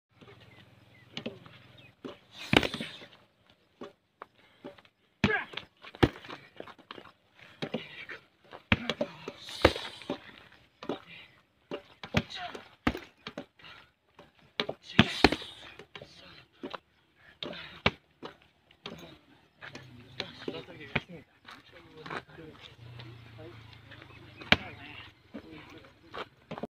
How to do squats with tyre
This is proper way of squats with weight or tyre